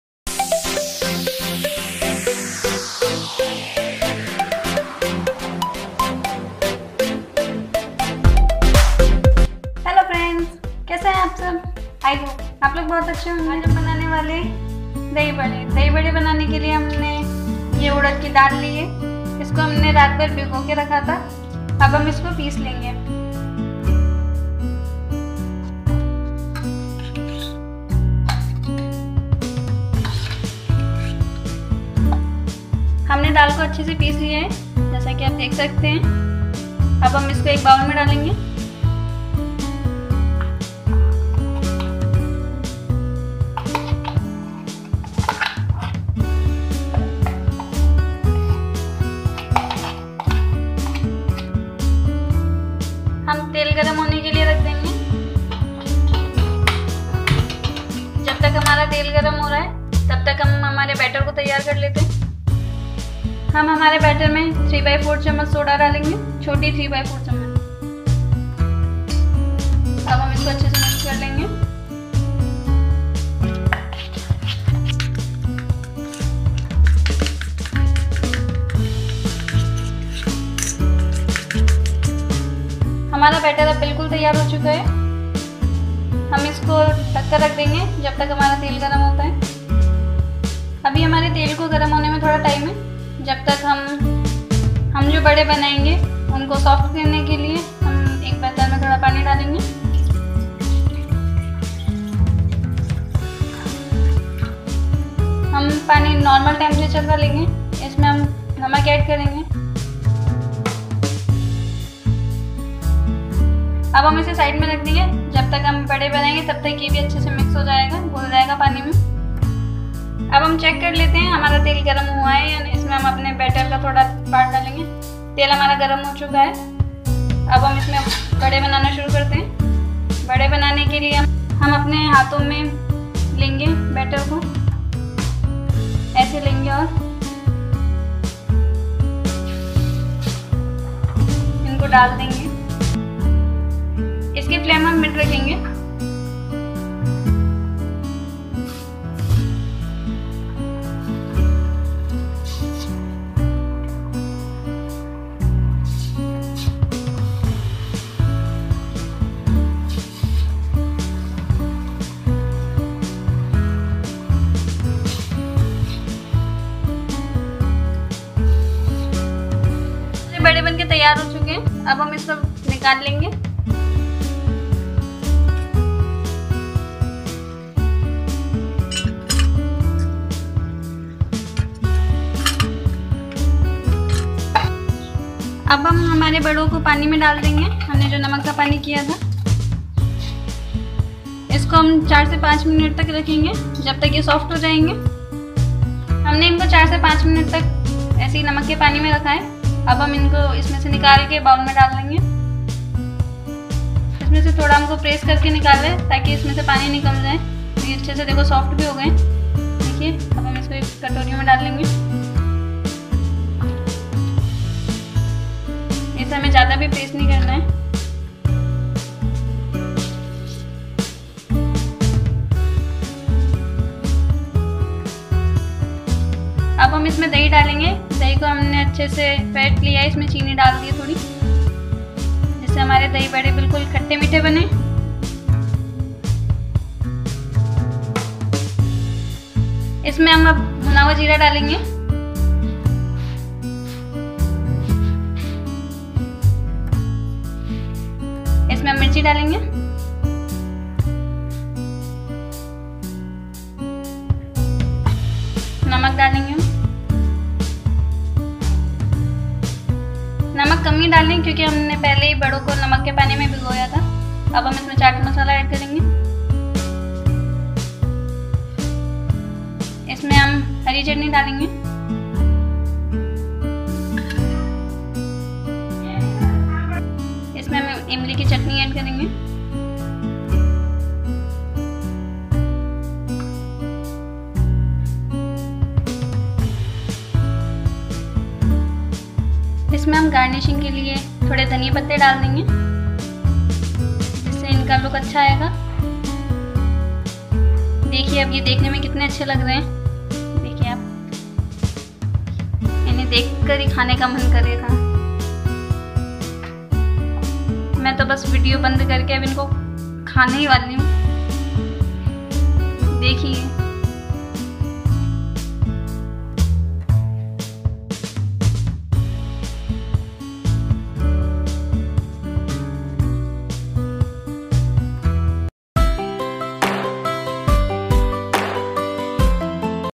dahi vada recipe in hindi, tastey dahi vada at home,dahi vada banane ka aasan trika, दही वड़ा
dahi vada recipe ingredients

1)dahi
2)dal ( उड़द)
3)lal mirch powder
4)namak
5)chat masala
6)imli ki chatni
7)hari chatni
8)baking powder
9)bhuna hua Jira